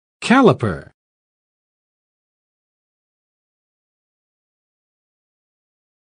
How to Pronounce Calliper
This video shows you how to say Calliper.